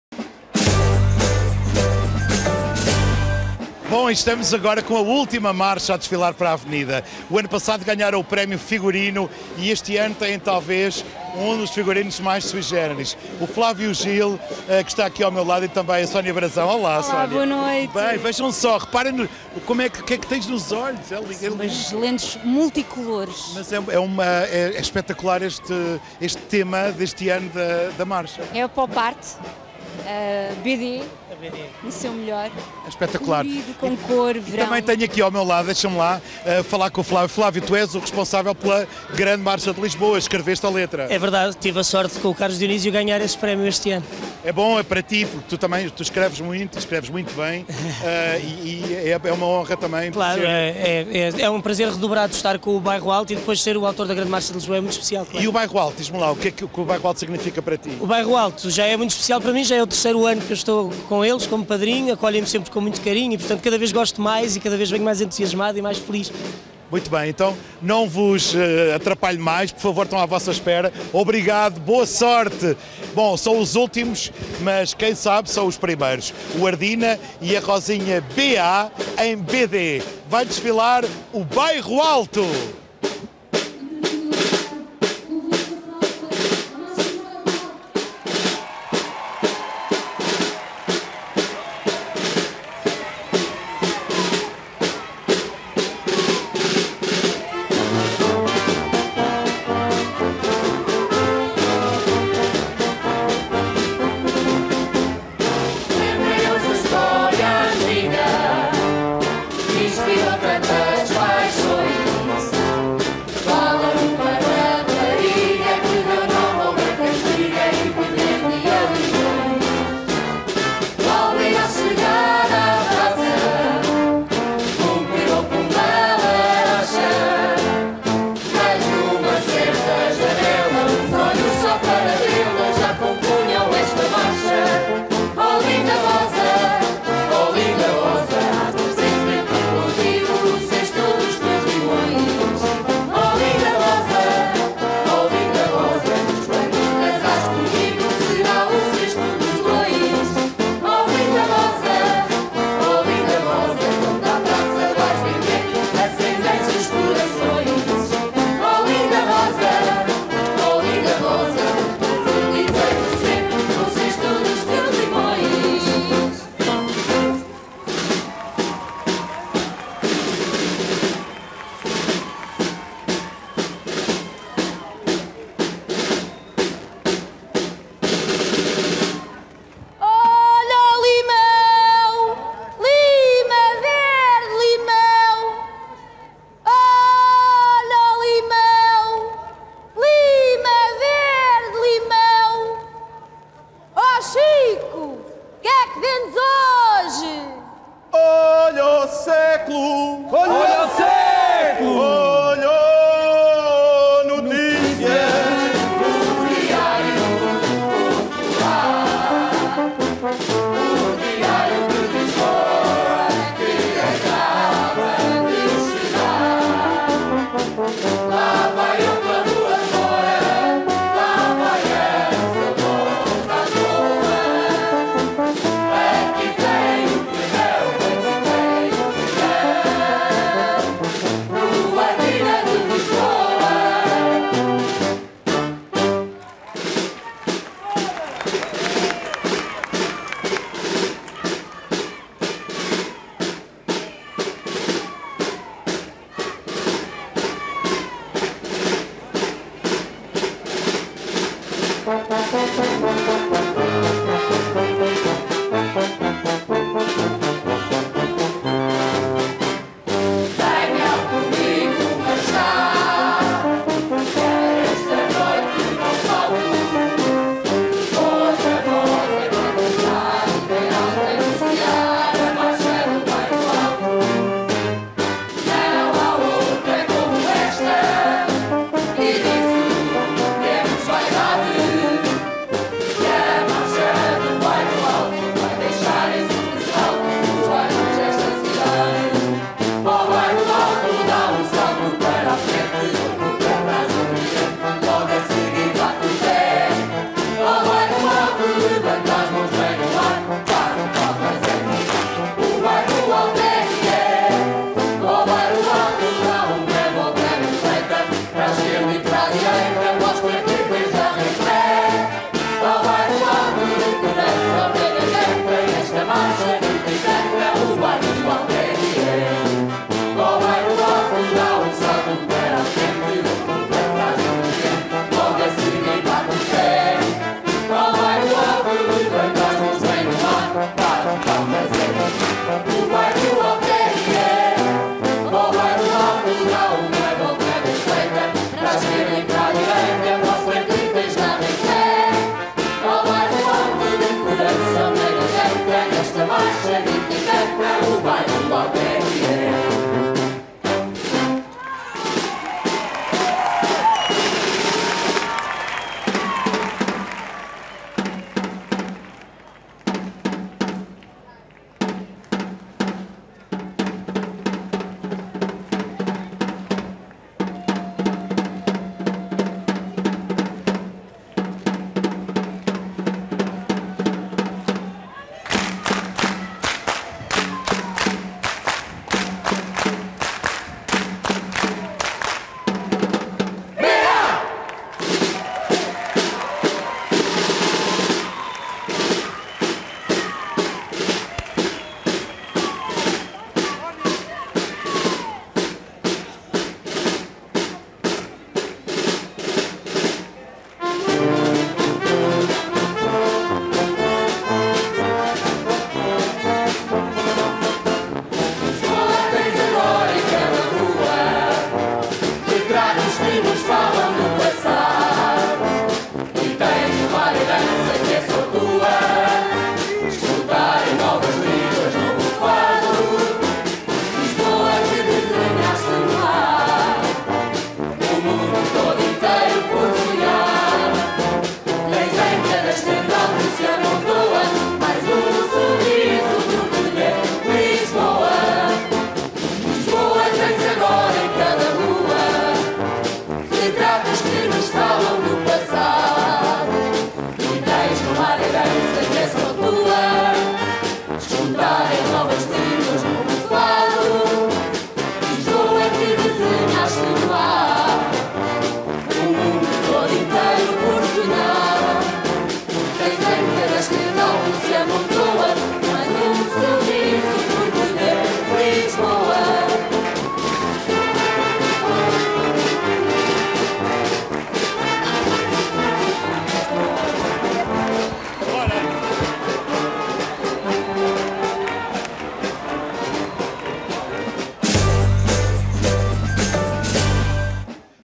Marchas 2017 Avenida - Marcha do Bairro Alto
Marcha do Bairro Alto

Tema: O Ardina e a Rosinha B.A em B.D.
Madrinha: Sónia Brazão
Padrinho: Flávio Gil
Organizada pelo Lisboa Clube Rio de Janeiro

Posição: 2º Marcha do Bairro Alto - 237 pontos
Prémio por Categoria: Melhor Desfile da Avenida

Classificação Final:

1º Marcha de Alfama - 247 pontos
2º Marcha do Bairro Alto - 237 pontos
3º Marcha da Madragoa - 236 pontos
4º Marcha de Alcântara - 232 pontos
5º Marcha da Bica - 229 pontos
6º Marcha de Carnide - 223 pontos
7º Marcha da Graça - 219 pontos
8º Marcha do Castelo - 209 pontos
9º Marcha de São Vicente - 208 pontos
10º Marchas da Penha de França e da Mouraria (ex-aequo) - 205 pontos
12º Marcha da Ajuda - 202 pontos
13º Marcha dos Olivais - 200 pontos
14º Marcha da Bela Flor-Campolide - 195 pontos
15º Marcha de Marvila - 191 pontos
16º Marcha de Campo de Ourique - 181 pontos
17º Marchas de Belém e do Alto do Pina (ex-aequo) - 180 pontos
19º Marcha de Benfica - 170 pontos
20º Marcha de Santa Engrácia - 163 pontos

DIREITOS:
Direitos de Imagem e Som: RTP - Rádio e Televisão de Portugal, S.A.